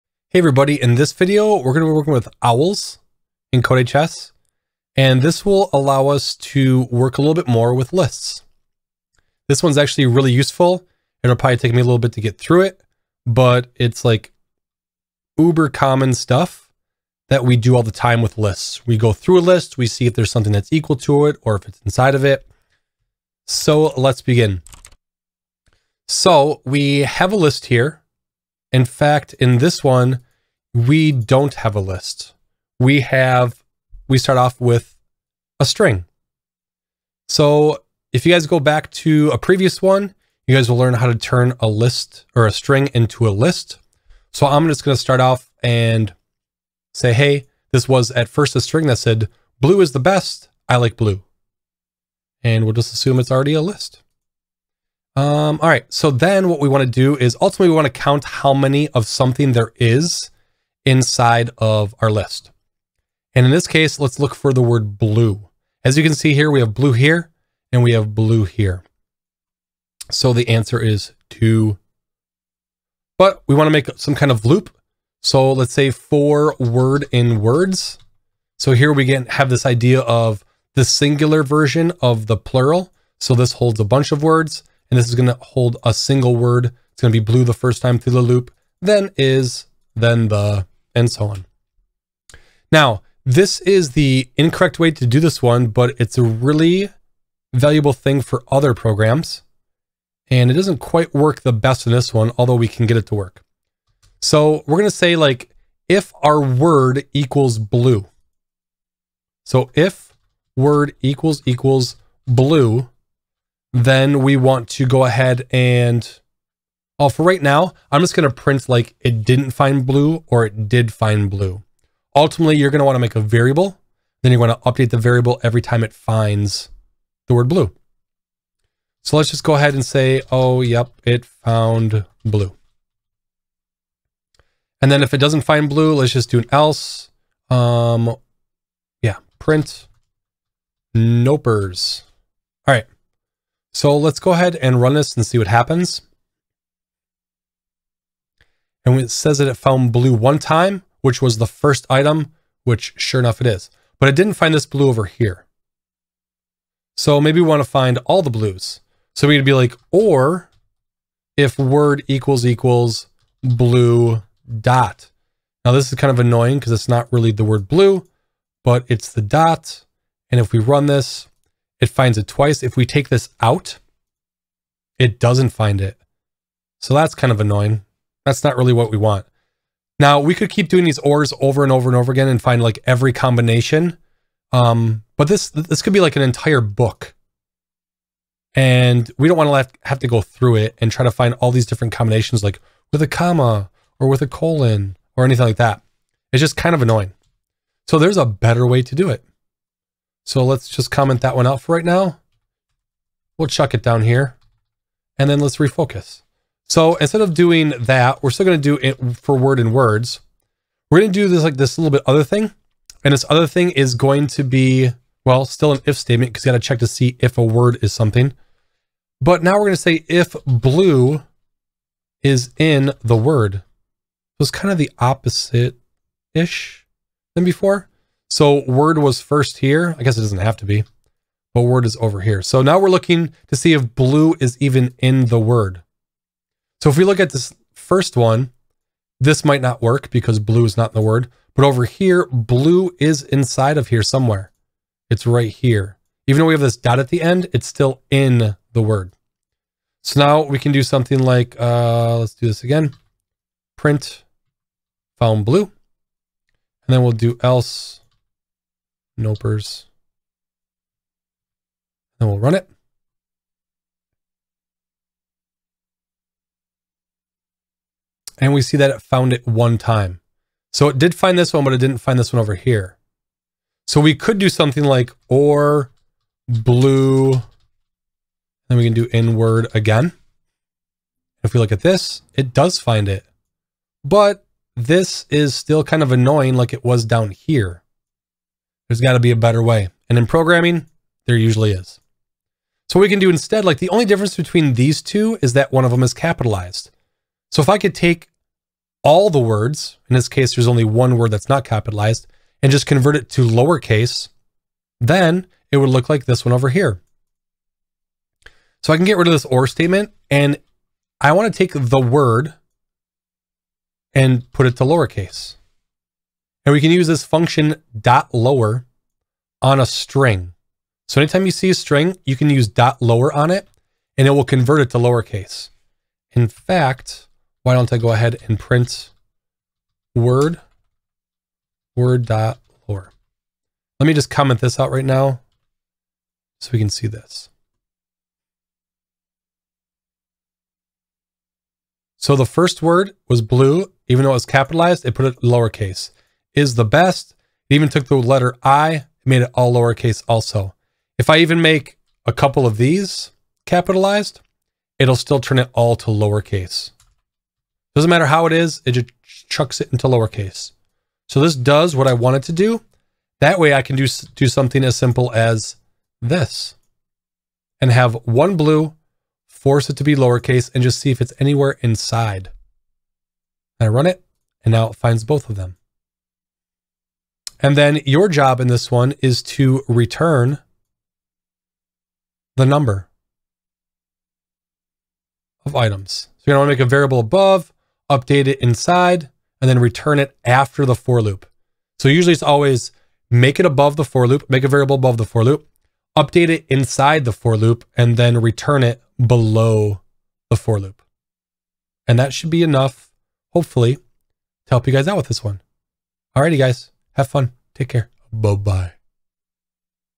CodeHS Python - Lists - Owls
CodeHS.com
Unit 5: Python Programming 1 and 2
Creating and Altering Data Structures
For Loops and Lists